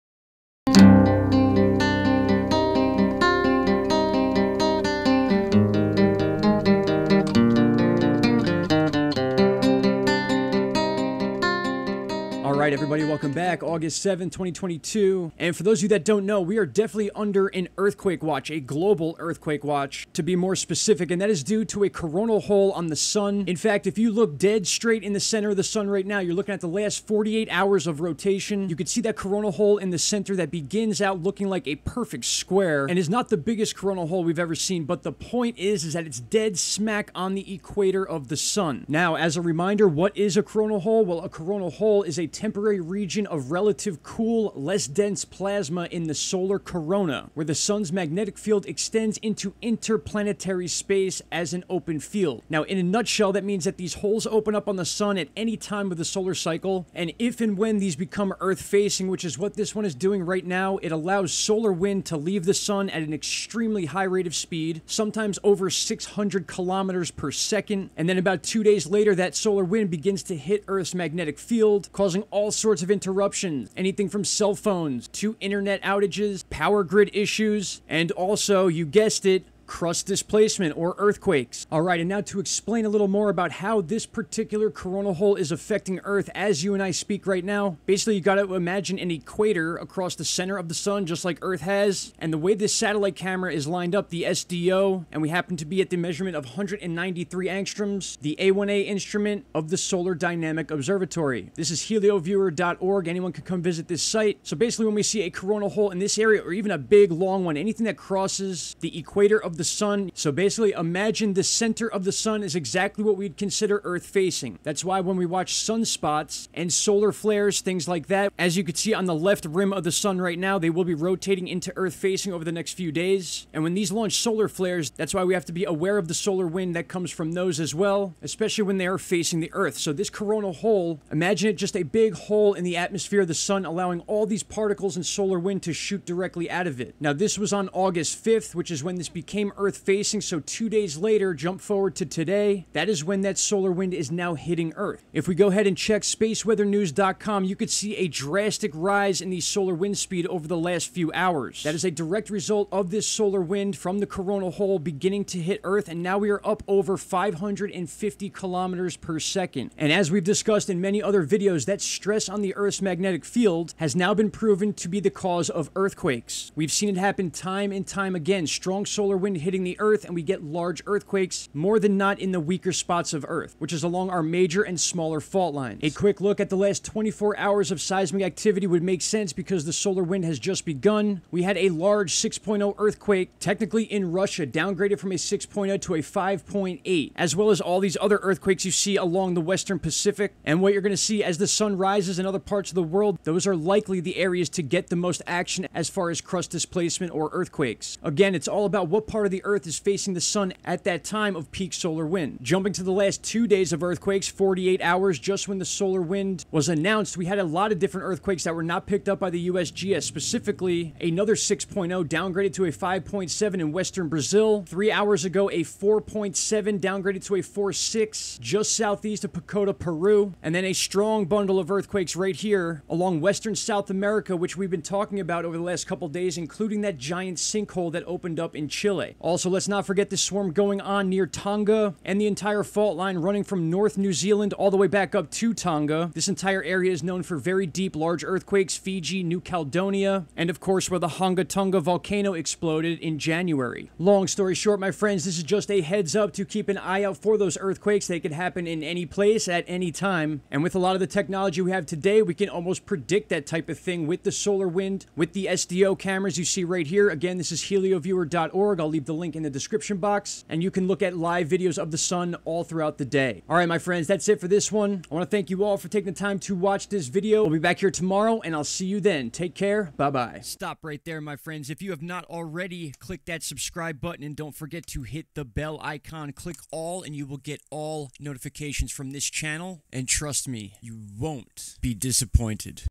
EARTHQUAKE Warning - LARGE Solar Storm NOW Impacting Earth "Geomagnetic Storm"
We are now under an Earthquake Watch for at least 24 hours as a Large 'Coronal Hole' released Solar wind at nearly 600KM per second that is now hitting Earths magnetic field. Video Discusses the cause and effects of such a situation!
*IF you would like To Support In2Thinair*